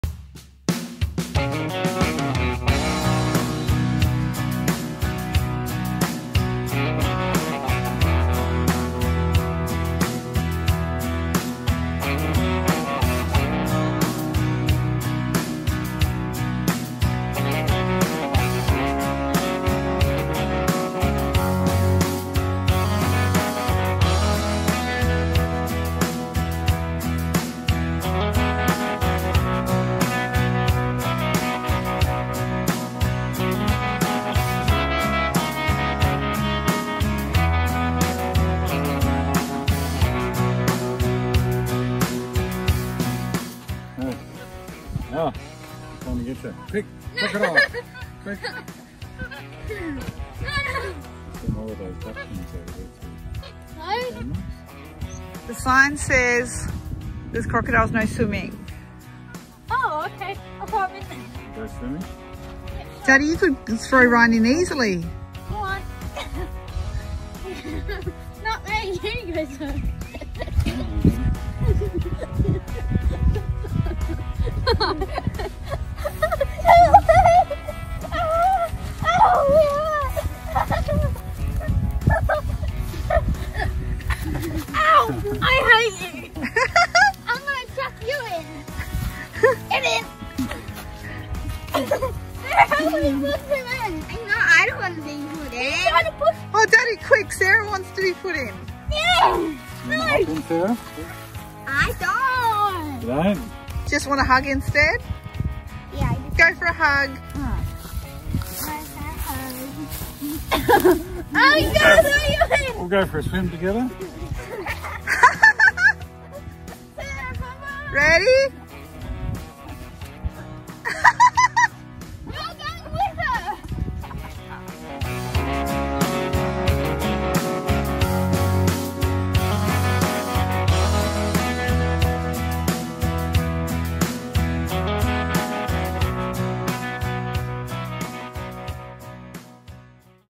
Sandy Billabong | Kakadu
Whilst in Kakadu we past a sign to Sandy Billabong so went in and had a look. 4WD track was good too. Dad tried to chuck me and Sarah in to the billabong full of crocs!